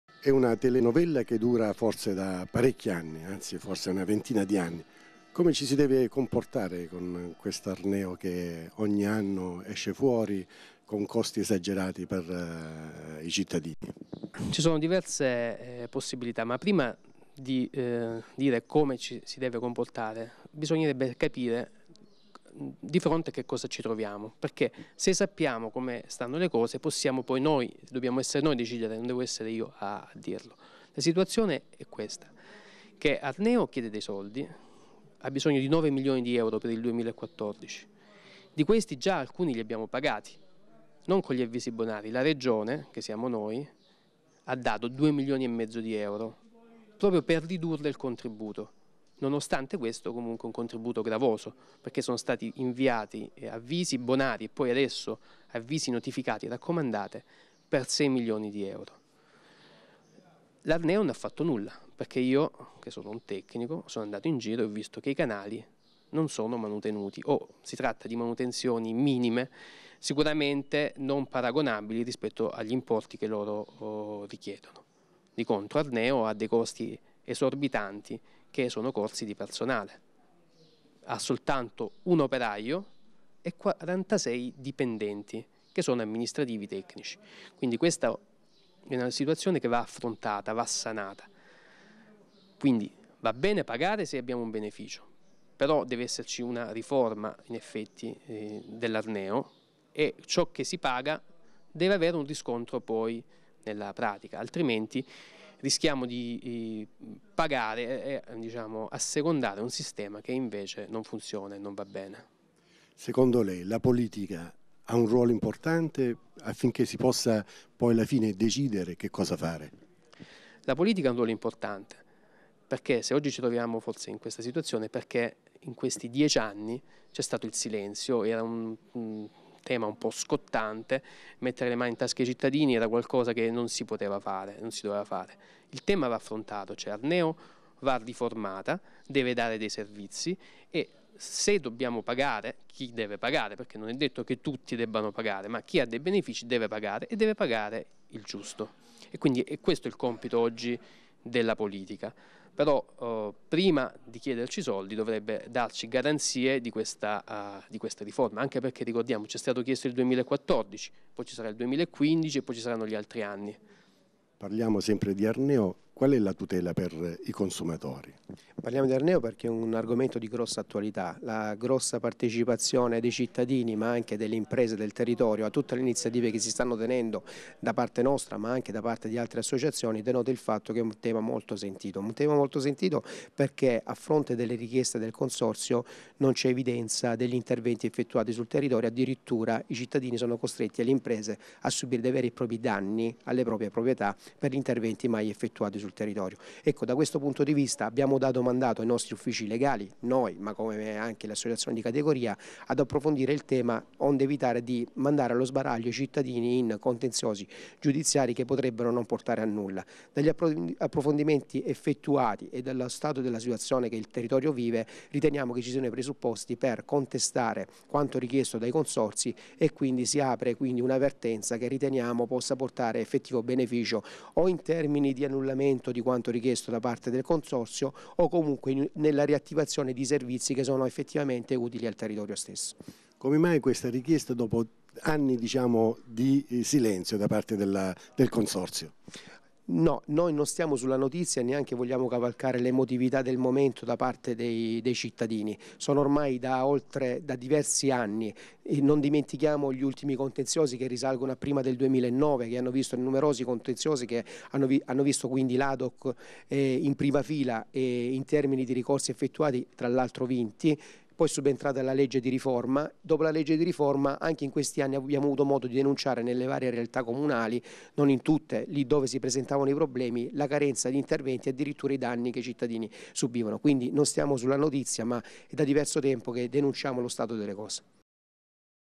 ARNEO..... Come tutelarsi!
In questi giorni migliaia di cittadini si sono visti recapitare avvisi di pagamento inviati dal consorsio di bonifica ARNEO, con i quali si invita al pagamento di tributi richiesti a fronte di attività che ha svolto il consorsio.
L'ADOC di Brindisi ha predisposto un modello di "instanza e di annullamento in autotutela ed interpello". Che potrà essere scaricato accedendo al sito o che potrà essere compilato presso le sedi ADOC. E' importante ricordare che il "ricorso" per annullamento in "autotutela" non sospende i termini assegnati per il pagamento.